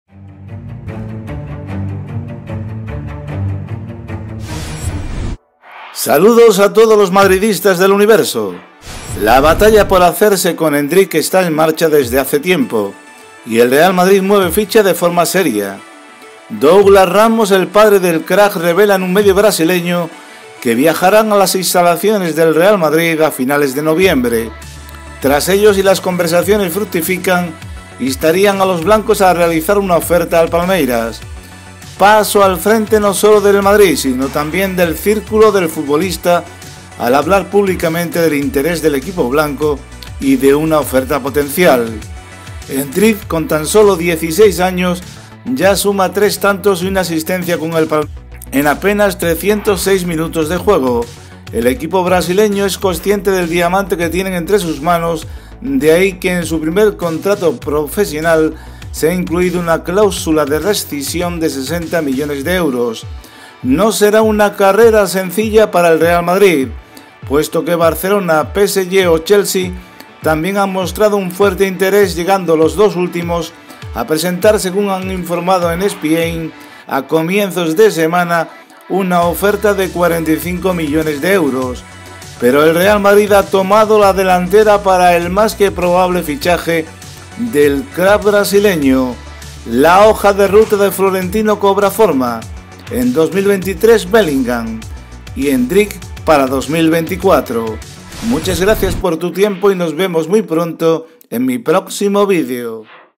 EL REAL MADRID SE ADELANTA A CHELSEA, PSG Y BARCELONA PARA FICHAR A ESTE SUPERDELANTERO
El representante del jugador llega a finales de este mes a España para iniciar las negociaciones.